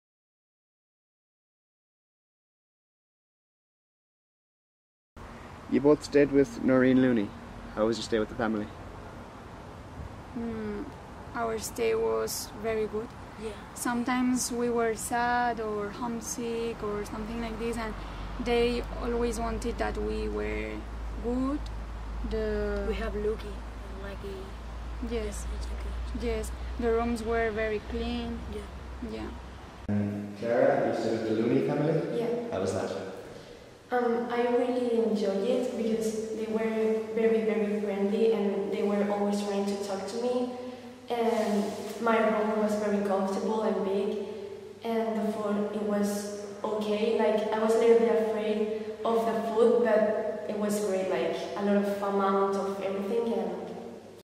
2018 loon nor
English language students (EFL)  of Killarney School of English Ireland describe home stay and staying with their Irish host family.